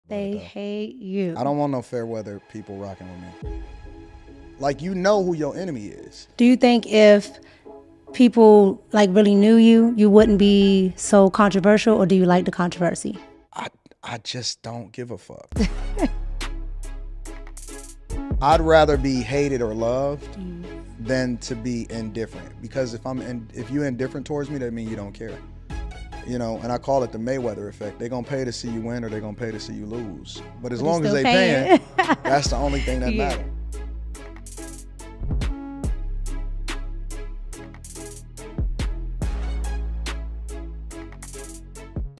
Anton Daniels Trailer| "The Red Cup Podcast Special Edition", Hosted
The Red Cup Podcast host@AntonDaniels@Popculturemicca , pours it up with one of the most loved and hated Youtuber's and content creator. @AntonDaniels is a controversial figure in the YouTube and red pill space. His work ethic is undeniable hence him being a viral sensation and his growth is unstoppable. Join "The Red Cup Podcast" as we do what we do best! Pour it up with those who influence, create, disrupt and make waves in pop culture and media now!
Join the Red Cup Mafia| Merch Coming Soon!